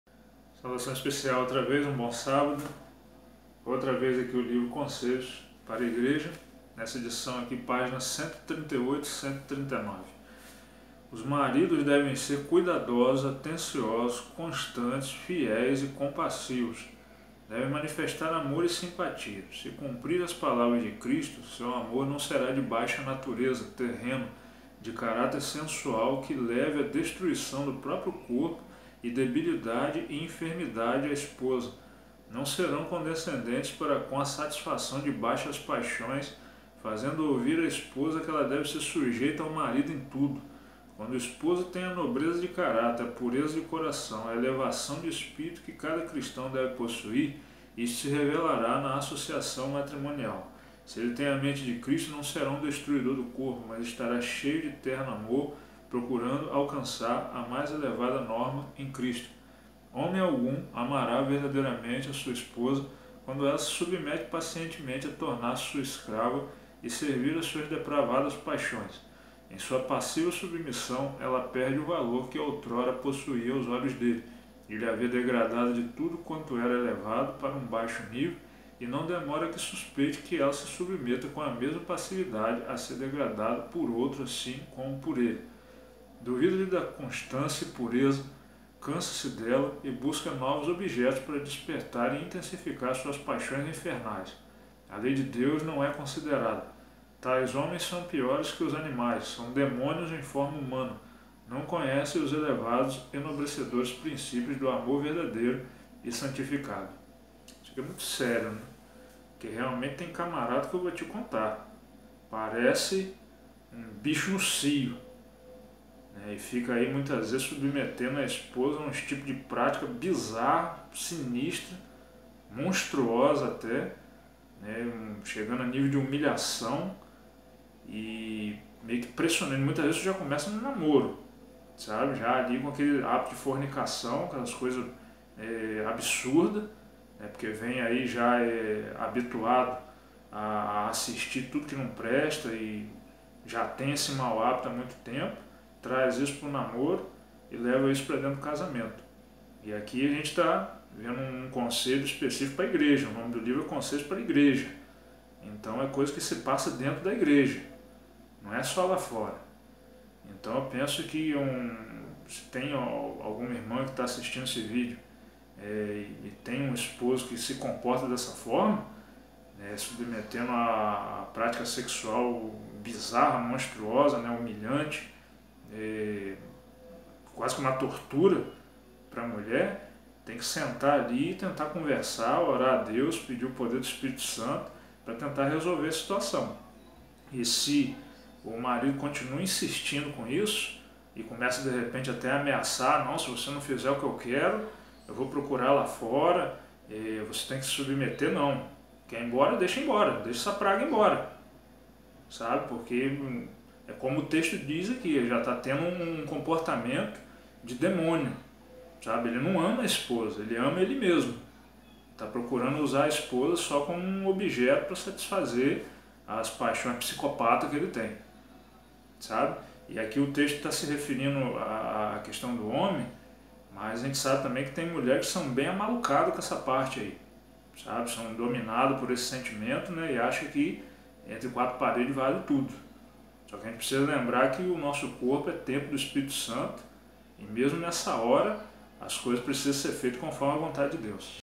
Demônios em forma humana
Mensagem Urgente

 Os maridos devem ser cuidadosos, atenciosos, constantes, fiéis e compassivos. Devem manifestar amor e simpatia. Se cumprirem as palavras de Cristo, seu amor não será de baixa natureza, terreno; de caráter sensual que leve à destruição do próprio corpo, e debilidade e enfermidade à esposa. Não serão condescendentes para com a satisfação de baixas paixões, fazendo ouvir a esposa que ela deve ser sujeita ao marido em tudo. Quando o esposo tem a nobreza de caráter, a pureza de coração, a elevação de espírito que cada cristão deve possuir, isto se revelará na associação matrimonial. Se ele tem a mente de Cristo, não será um destruidor do corpo, mas estará cheio de terno amor, procurando alcançar a mais elevada norma em Cristo. [139] Homem algum amará verdadeiramente a sua esposa quando ela se submete pacientemente a tornar-se sua escrava, e servir a suas depravadas paixões. Em sua passiva submissão, ela perde o valor que outrora possuía aos olhos dele. Ele a vê degradada de tudo quanto era elevado, para um baixo nível; e não demora a que suspeite que ela se submeta com a mesma passividade a ser degradada por outro assim como por ele. Duvida-lhe da constância e pureza, cansa-se dela, e busca novos objetos para despertar e intensificar suas paixões infernais. A lei de Deus não é considerada. Tais homens são piores que os animais; são demônios em forma humana. Não conhecem os elevados, enobrecedores princípios do amor verdadeiro e santificado. ( Conselhos para a igreja pag 138 e 139 )